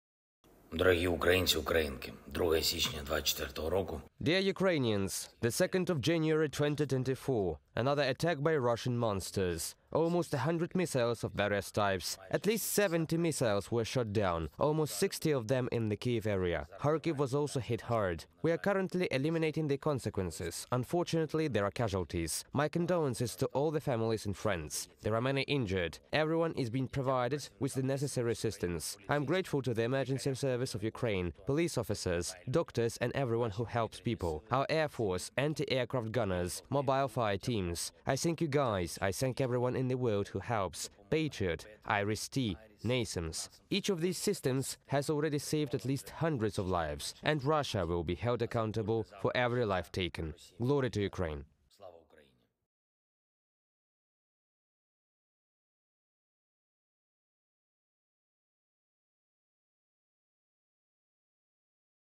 Russian Terrorist Strike with 90 Missiles Against Ukraine. Zelenskyy's Reaction
The year 2024 started with massive missile and drone attacks by Russian terrorists against the civilian infrastructure of Ukraine. Dozens are injured, and there are fatalities. 'Russia will answer for every life taken,' – Zelenskyy stated.